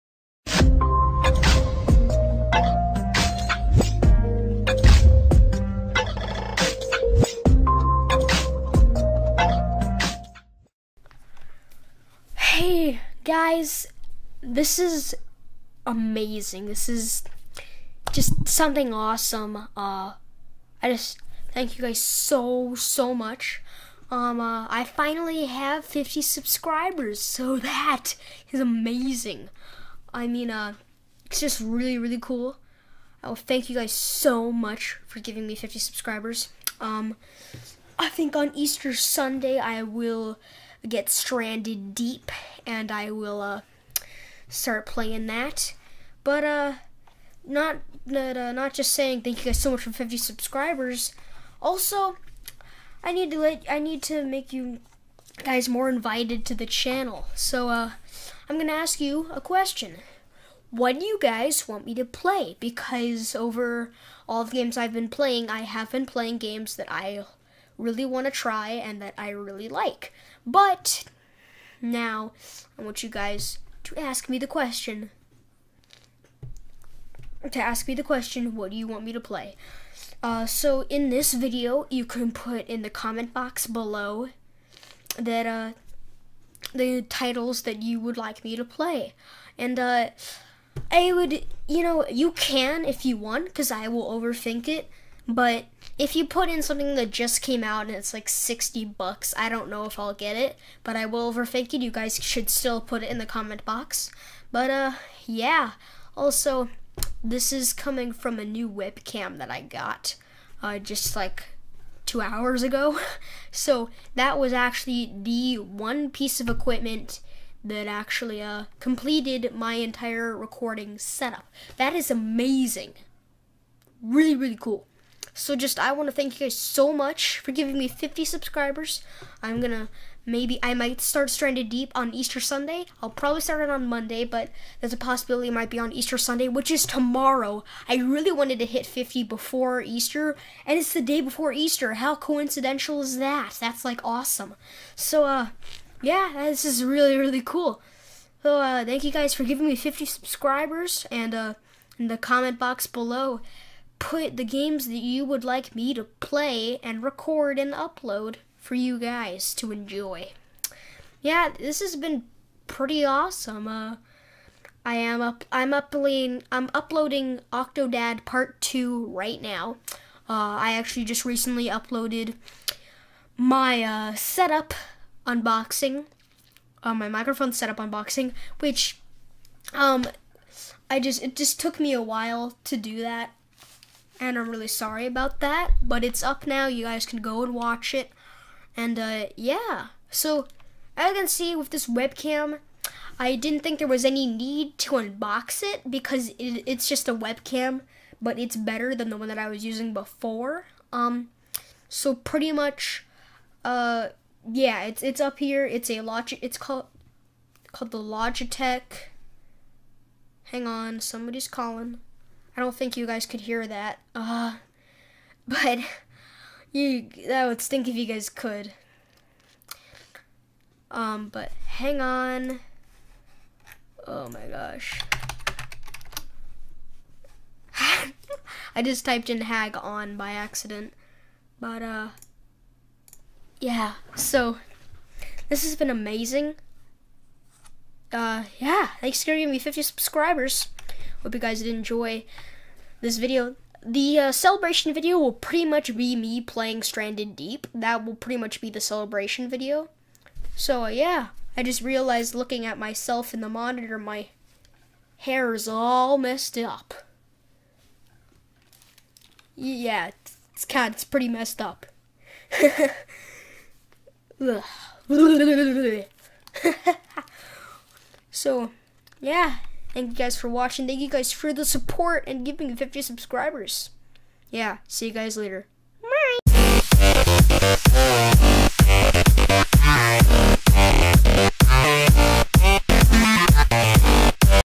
50 Subscriber Anouncement! + What Games Do You Guys Want Me To Play?
In this video I have to thank you guys, my awesome fans and viewers!  Also, what game do any of you want me to play?